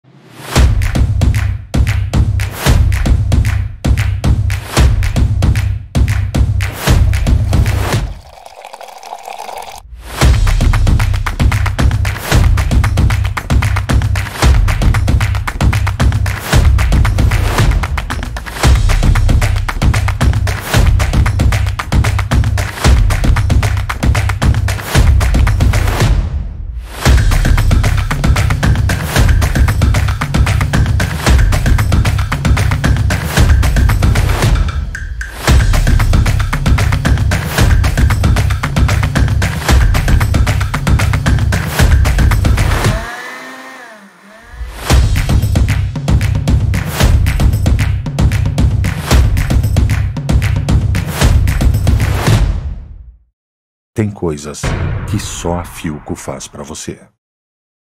Multiprocessador Philco PMP1600P Glass Turbo 9 em 1
Tudo o que você precisa para preparar suas receitas, você tem em um único equipamento! O Multiprocessador Philco PMP1600V Glass Turbo 9 em 1 1400W, traz a você grande praticidade na hora de cortar, processar, picar, liquidificar, fatiar, bater, ralar, espremer frutas, entre outros. São diferentes funções e a potência que você precisa em sua cozinha!